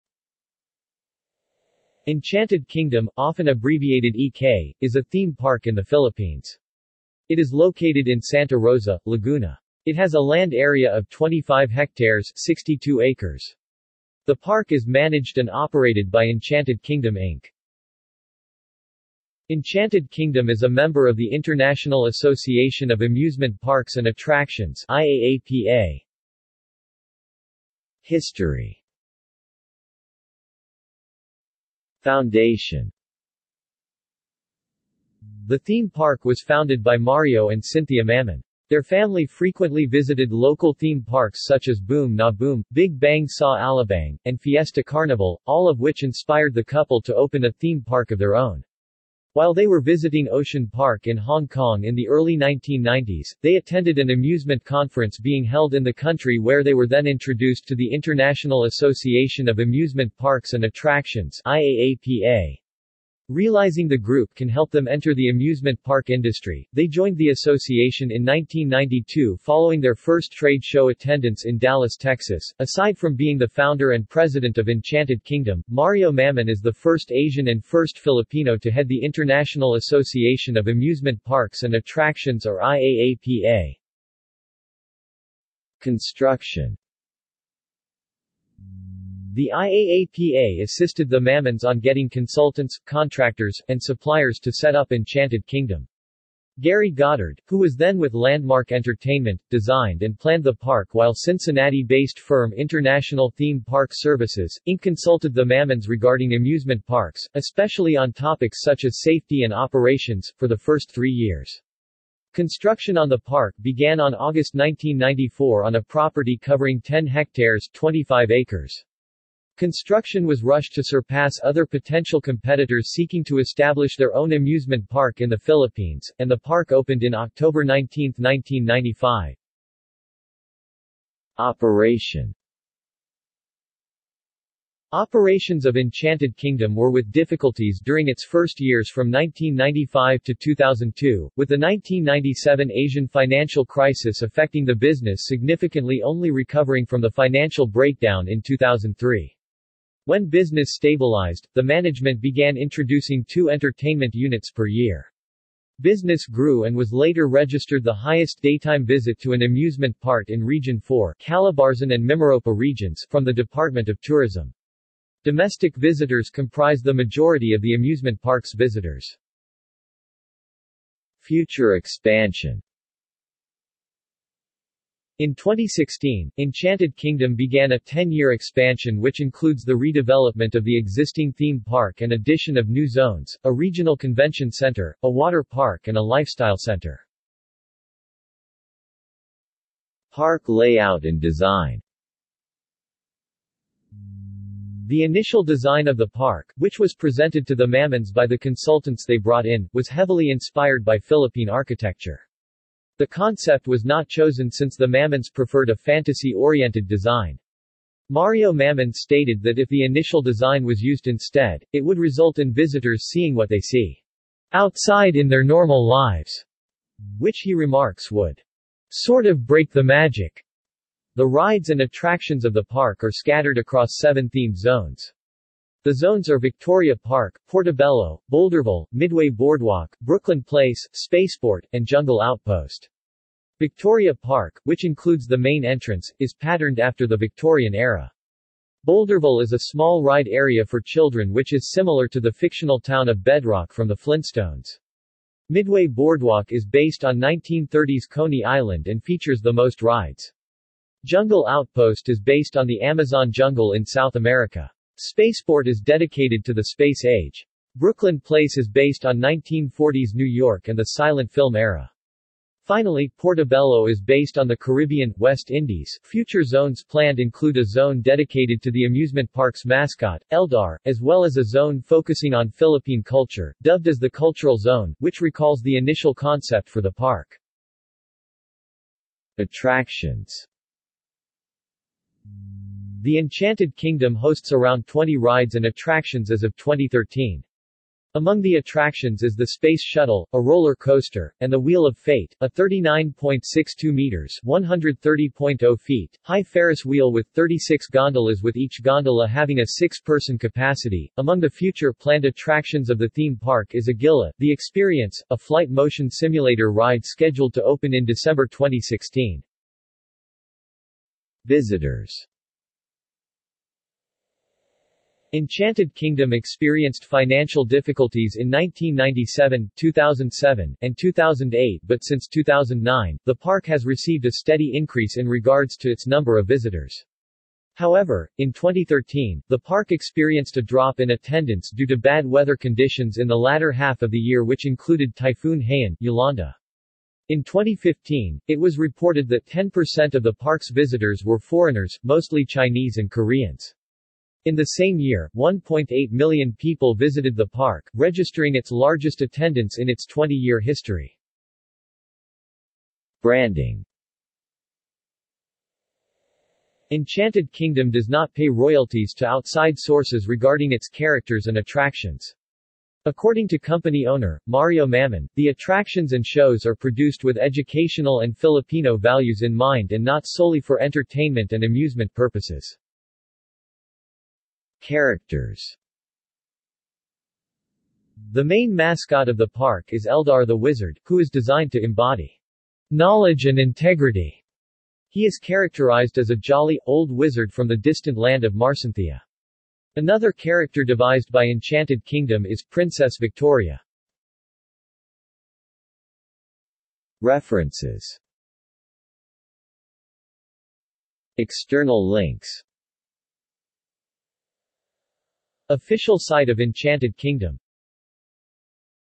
Enchanted Kingdom | Wikipedia audio article
This is an audio version of the Wikipedia Article:
Enchanted Kingdom

SUMMARY
Enchanted Kingdom, often abbreviated EK, is a theme park in the Philippines. It is located in Santa Rosa, Laguna. It has a land area of  25 hectares (62 acres). The park is managed and operated by Enchanted Kingdom Inc.
Enchanted Kingdom is a member of the International Association of Amusement Parks and Attractions (IAAPA).